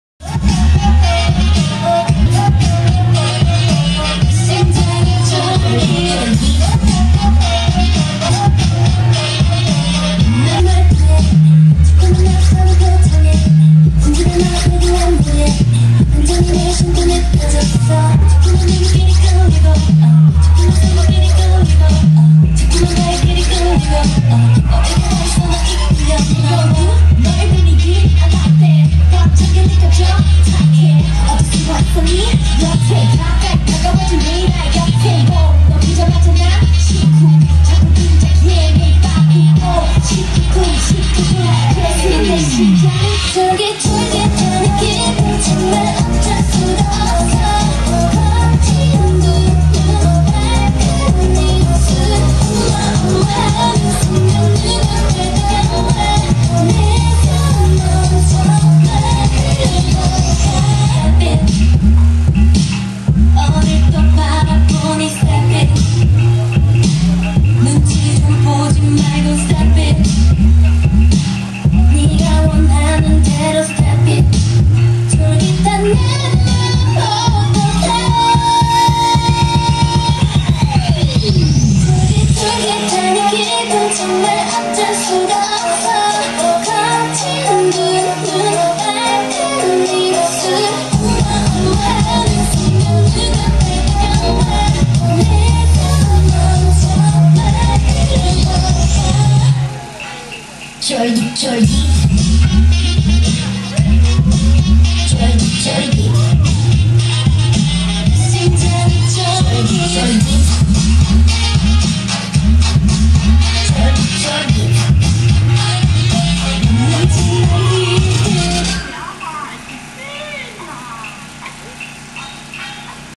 2017.10.29 台南 安南區 溪頂寮 吳家 進香大典 跳跳糖 Pop Candy 表演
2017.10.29 台南 安南區 溪頂寮 吳家  中壇三太子 往 學甲慈濟宮 進香大典